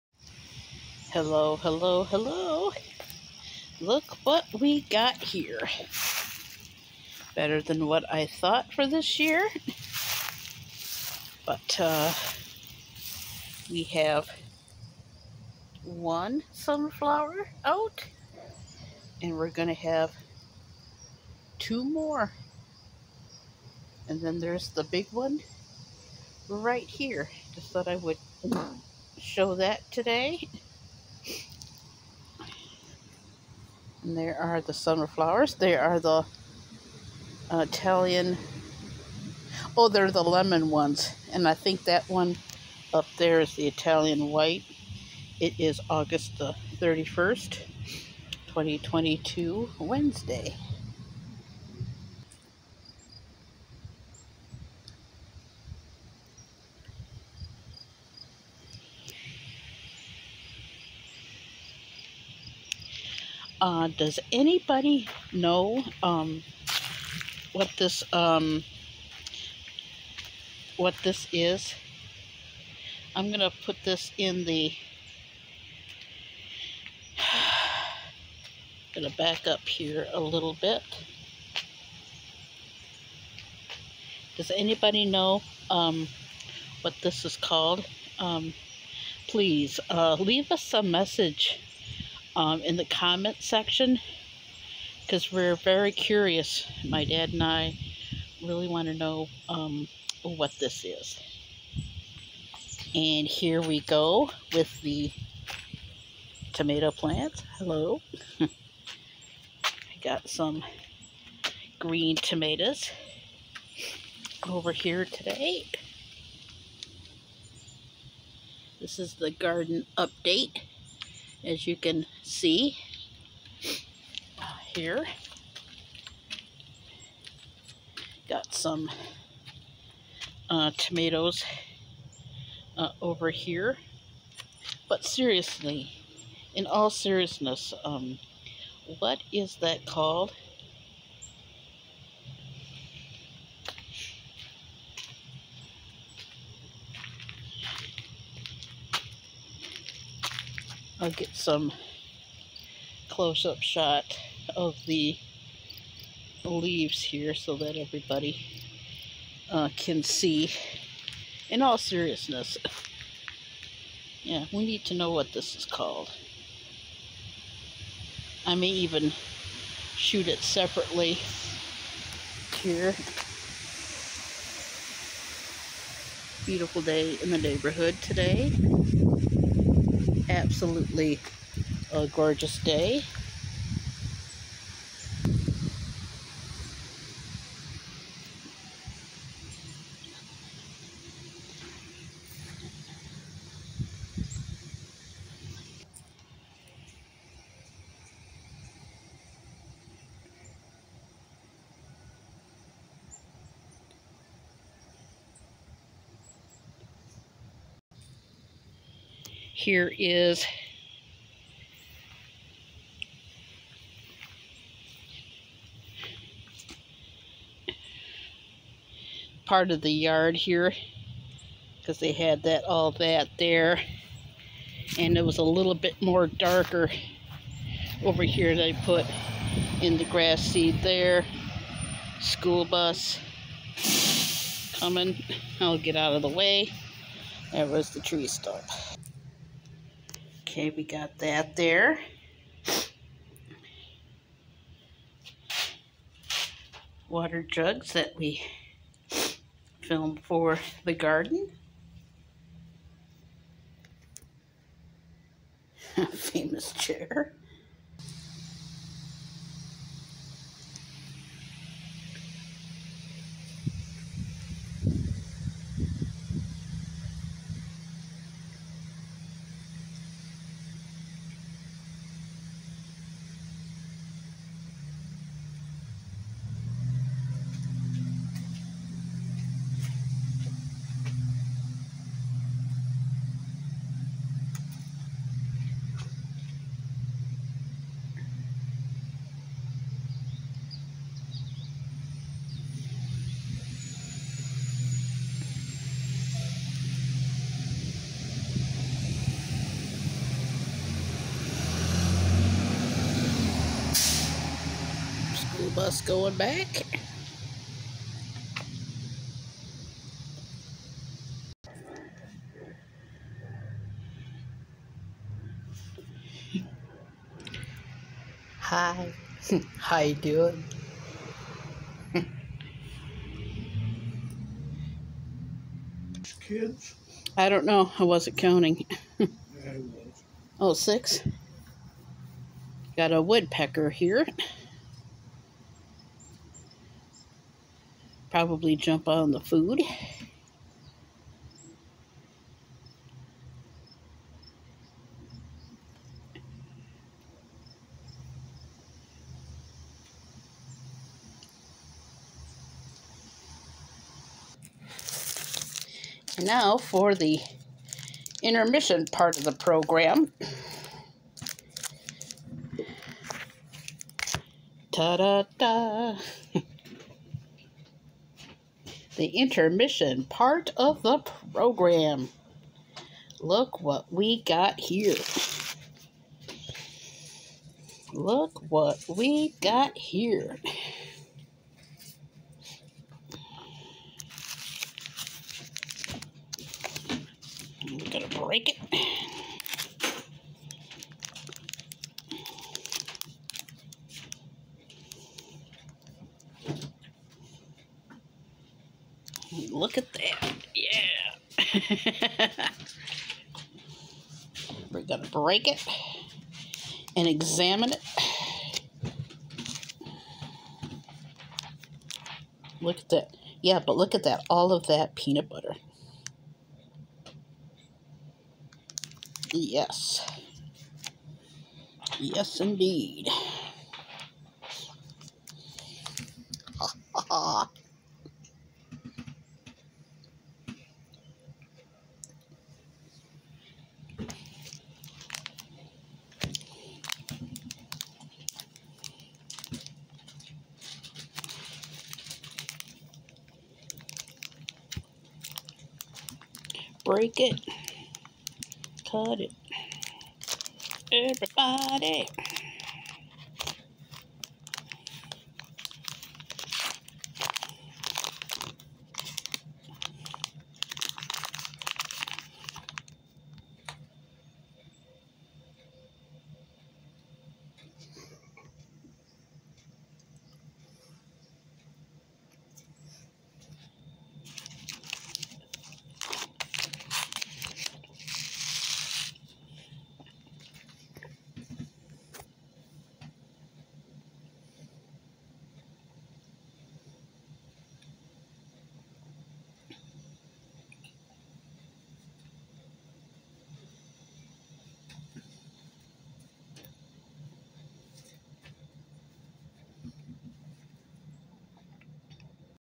garden update what's this and us silly 😜 8 31 2022 wed
print in the comments if you know what the long tall tree is in our patio 😁😂 enjoy print time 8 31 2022 wed enjoy your day